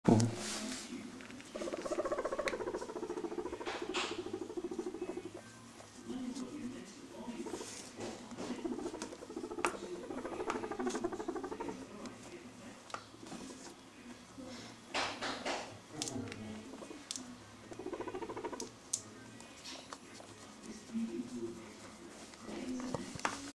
Guinea pig purring
Purring is usually associated with being content or happy. You will most likely hear it when you are gently petting your pig. However, sometimes when they hear a startling noise or suddenly feel threatened, they will vocalize this same noise but in short spurts.